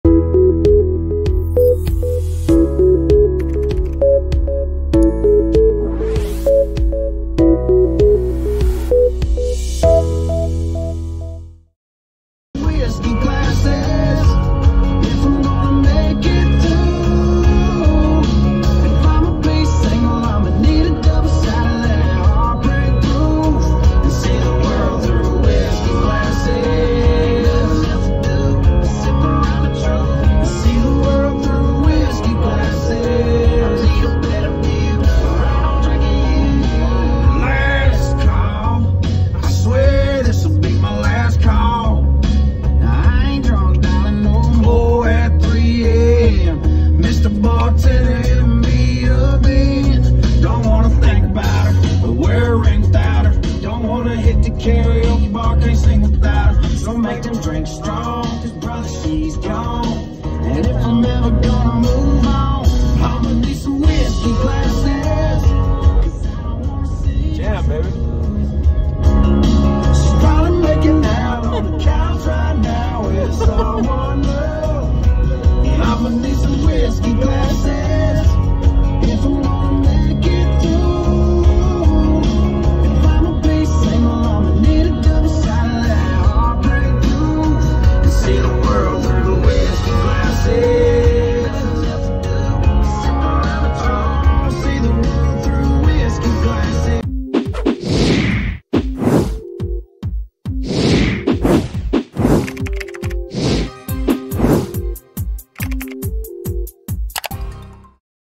What kind of music do NFL players listen to? *15 hour long car drive*
Ask me questions in the comments below or on my instagram @kamalei_correa